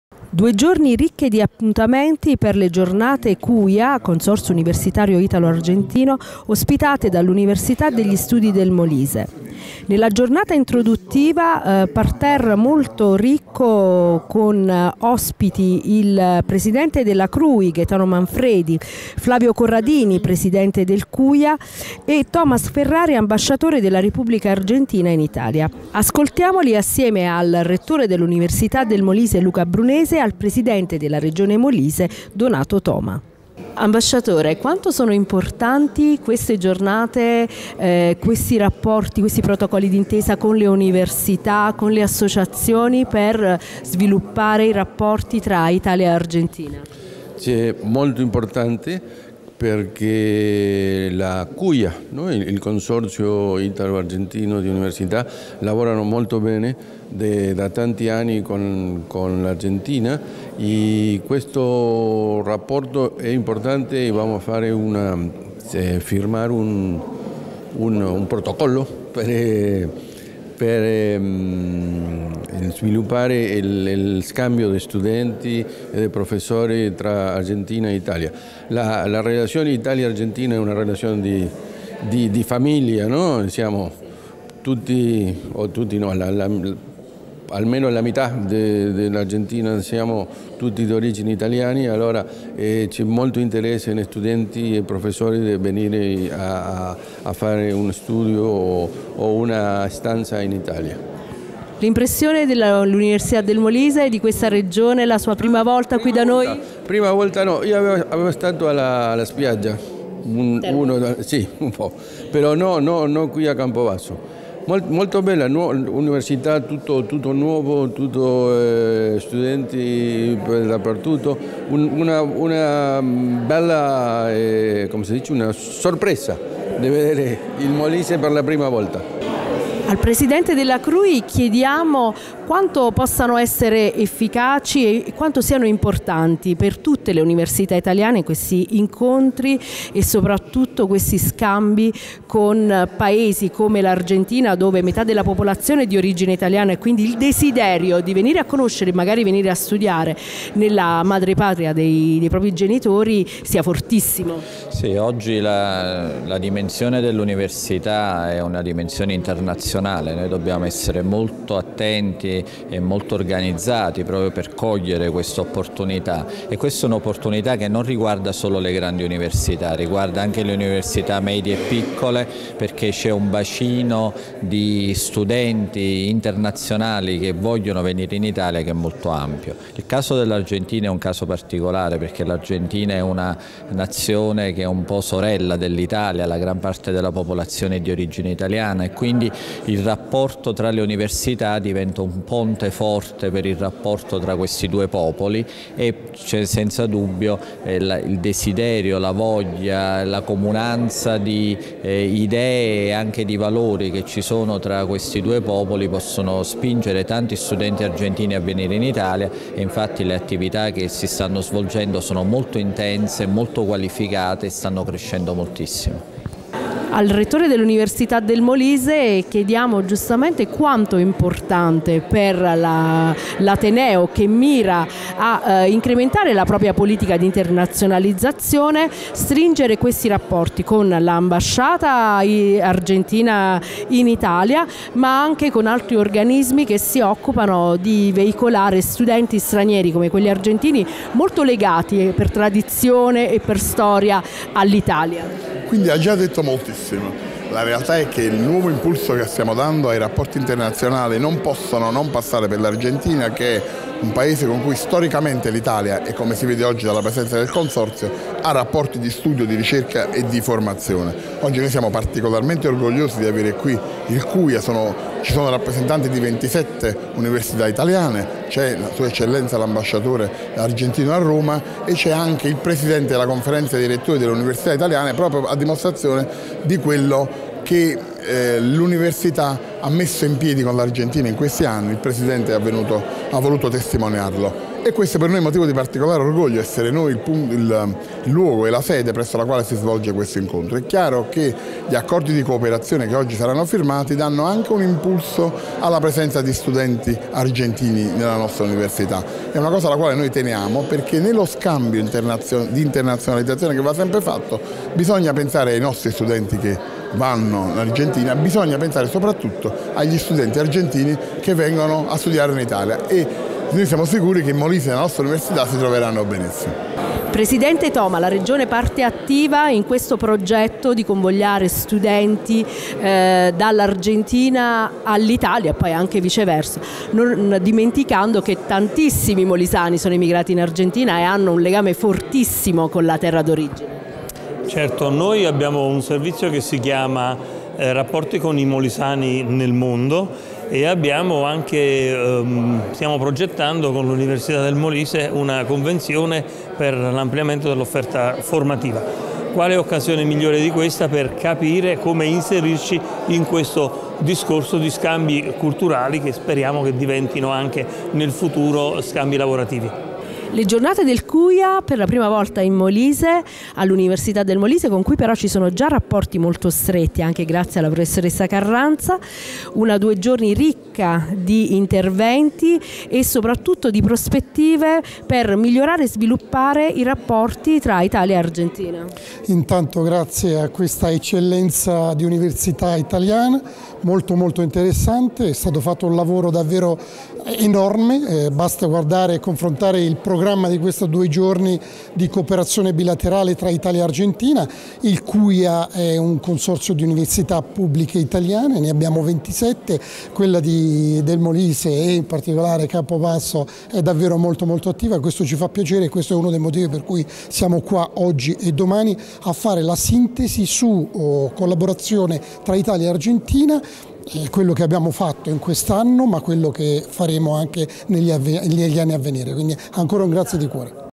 Giornate del CUIA
UniMol sempre più Internazionale. 11 e 12 ottobre 2019 le Giornate del CUIA, Consorzio Universitario Italia-Argentina con la partecipazione dell’Ambasciatore della Repubblica Argentina in Italia ed il Presidente della CRUI.